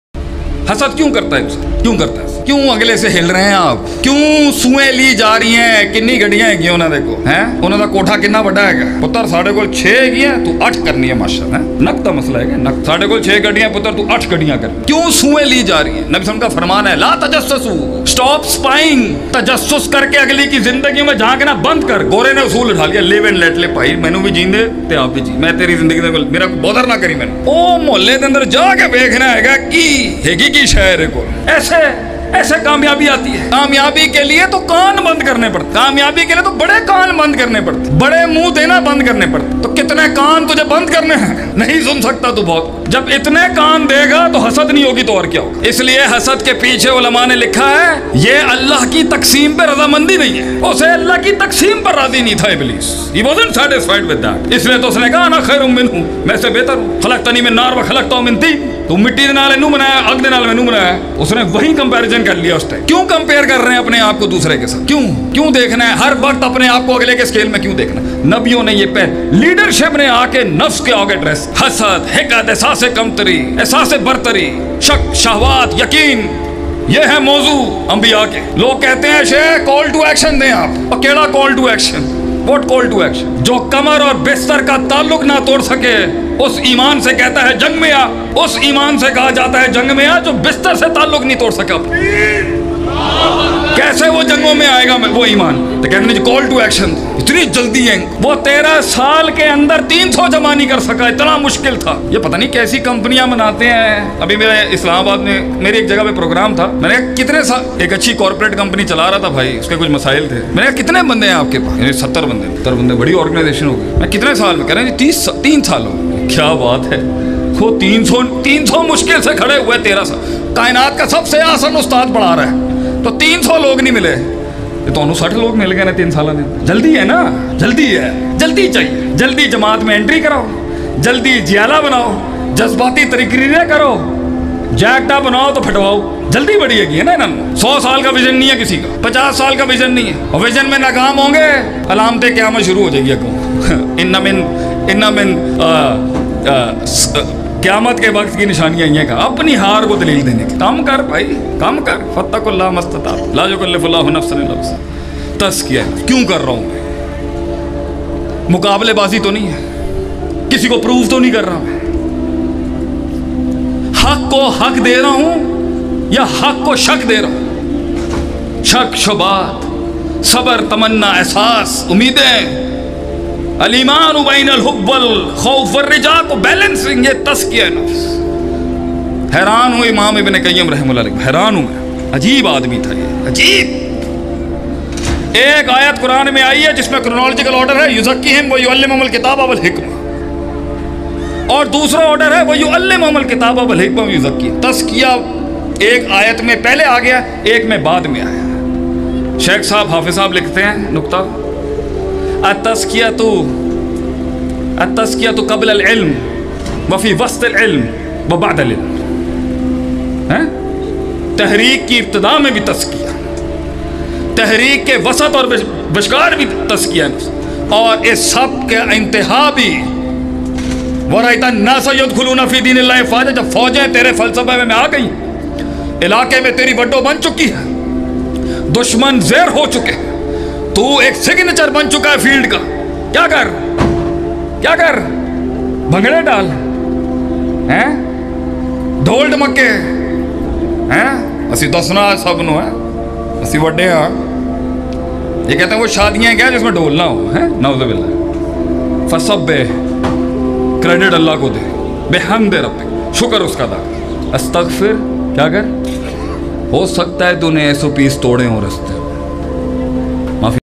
Beta Dafan Kr Apny Ap Ko || Sheikh Atif Ahmed || Motivational session by Shaykh Atif Ahmed
Bismillahir Rahmanir Rahim, Assalam o Alaikum Topic: Beta Dafan Kr Apny Ap Ko *Speaker: Shaykh Atif Ahmed